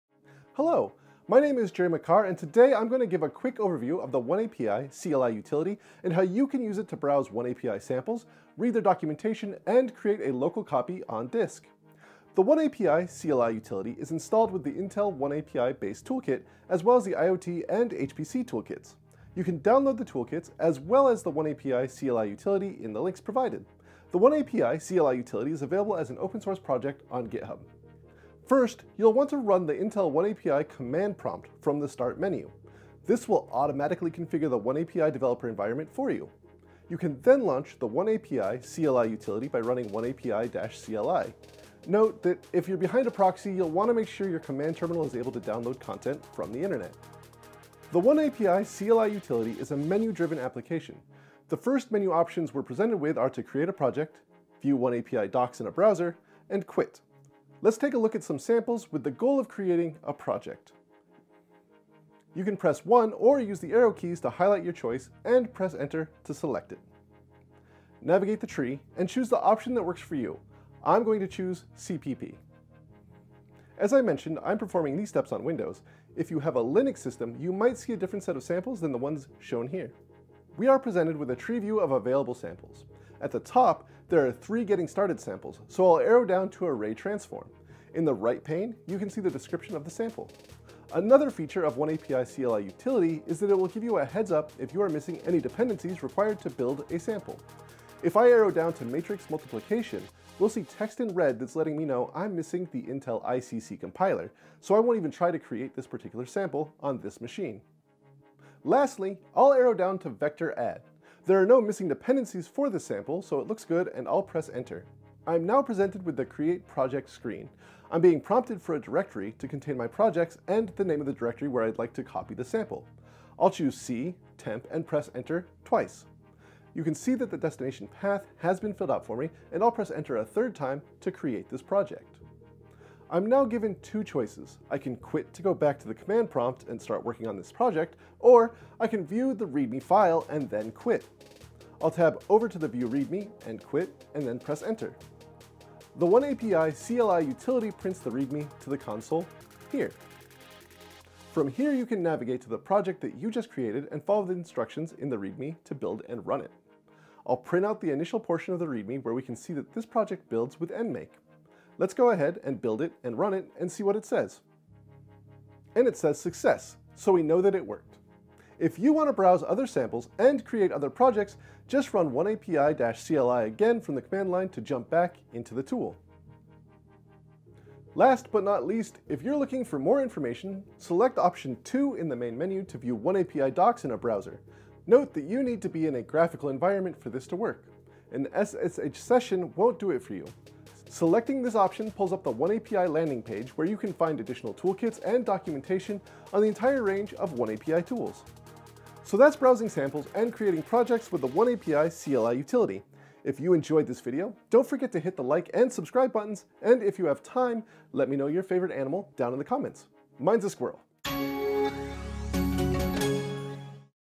Exploring oneAPI Samples from the Command Line | Intel Software
Get a quick overview of the oneAPI CLI utility and how you can use it to browse oneAPI samples, read documentation, and create a local copy on disk.

Written by John Hines